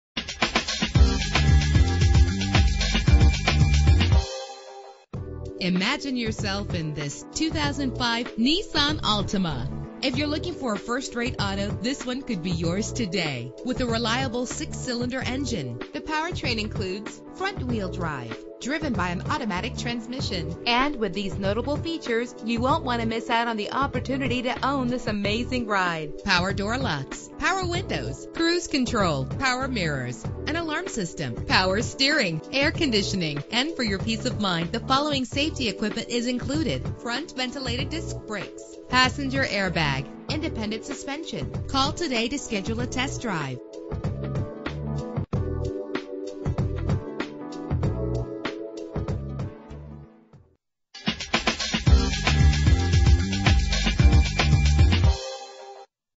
2005 Nissan Altima - Gonzales LA
Year: 2005
Make: Nissan
Model: Altima
Trim: 3.5 SE
Engine: 3.5 liter 6 cylinder 24 valve
Transmission: 4-Speed Automatic
Color: Maroon
Mileage: 98400
Address:
14295 Airline Hwy